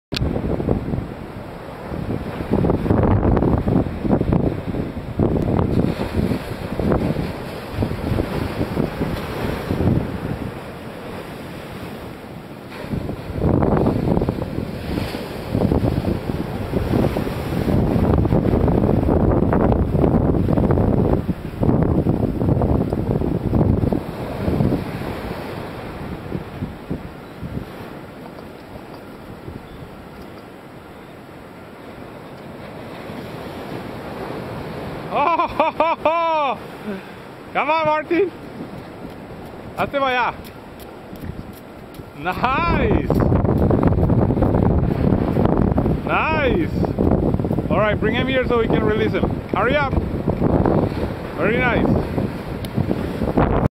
Jack crevalle rough seas
One more time Airnuts and Martin catching Jacks from shore, !! WAY TO GO MARTIN !!!